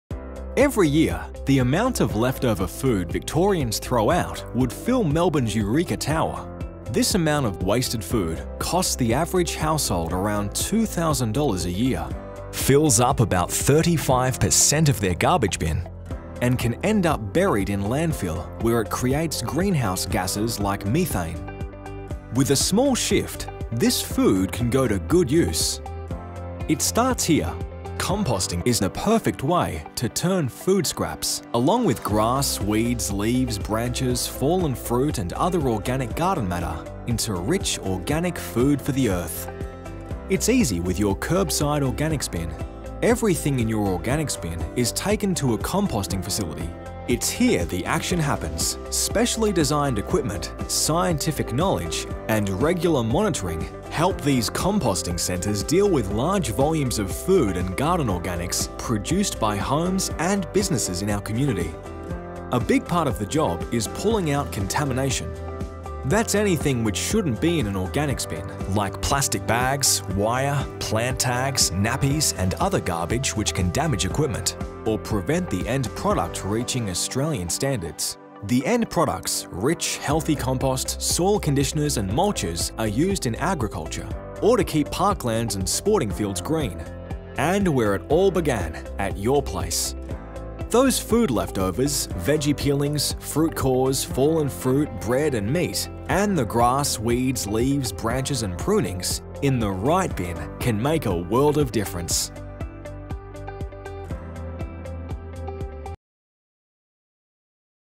Video on what happens to organics
A video that shows what really happens to organics (garden and food waste) after it gets emptied from your bin. Produced for North East Waste and Regional Resource Recovery Group, Victoria, Australia by Mesch Engagement and Mad Dog Productions